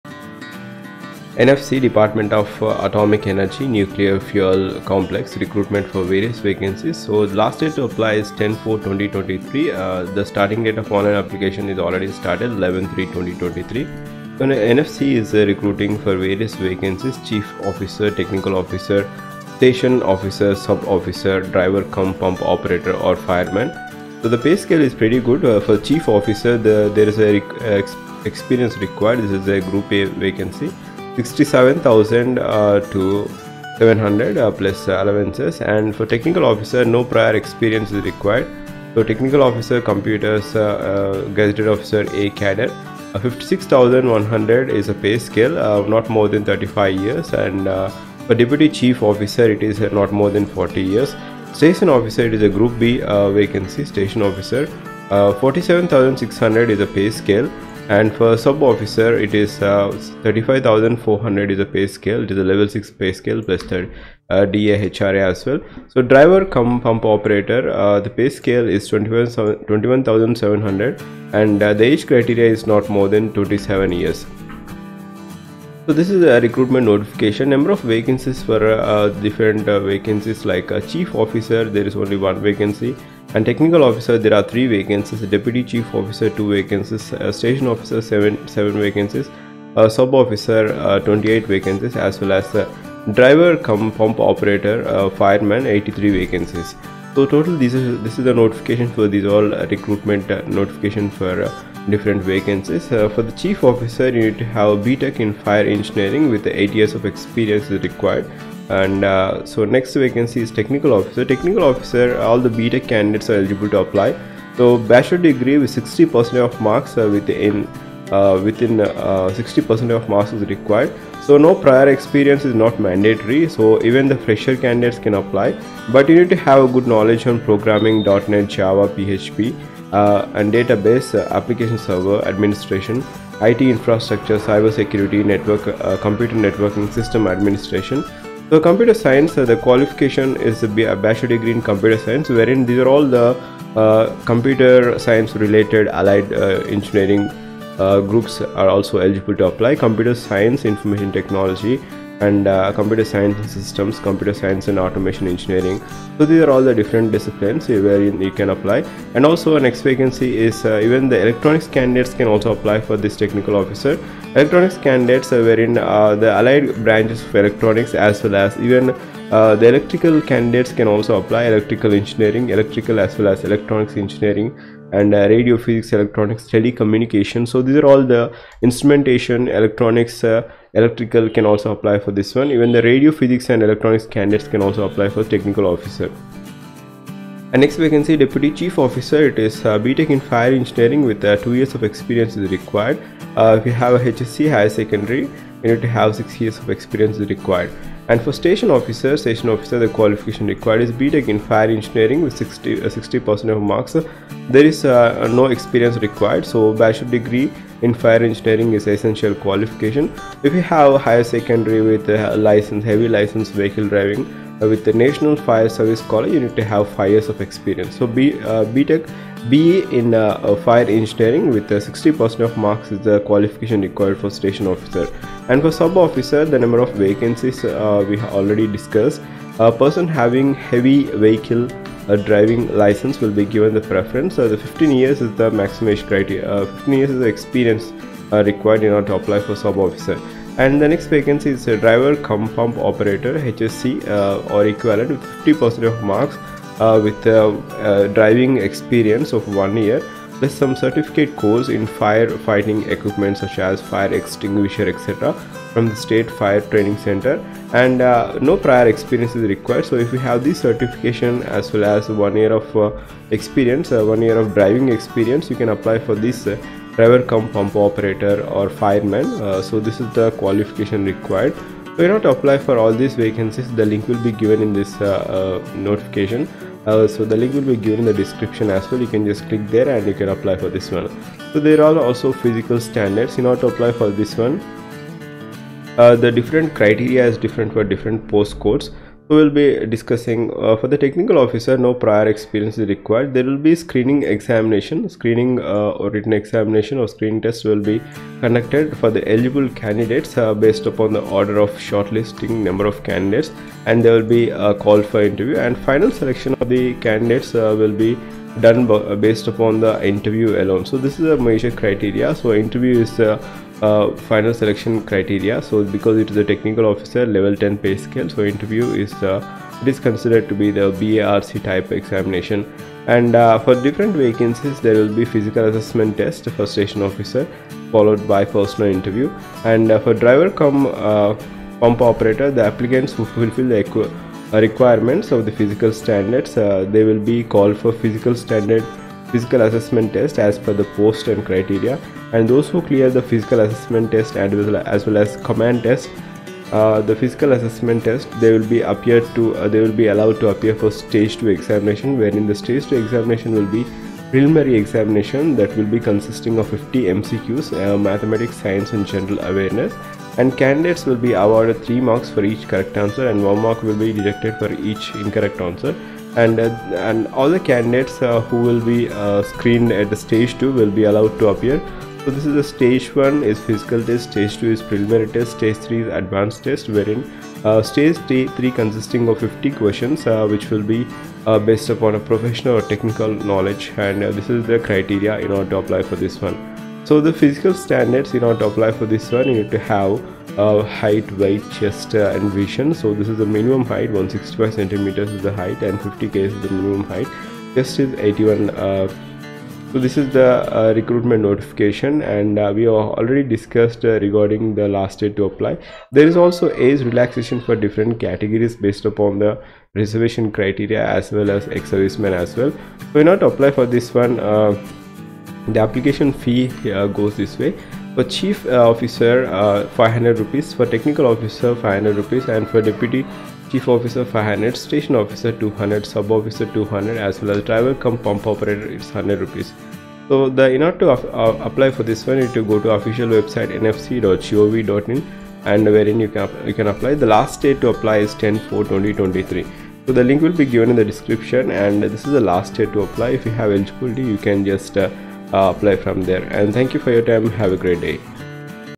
NFC Recruitment 2023 jobalert|Freshers|Job News|Jobs|Job vacancy 2023|Latest Job👍😍☹️💕
Nuclear Fuel Complex
Department of Atomic Energy
(NFC)
Recruitment 2023
Salary : Rs.  56,100 /- PM
Job Vacancy 2023, Jobs for Freshers 2023
 |Freshers|Job vacancy 2023|Latest Job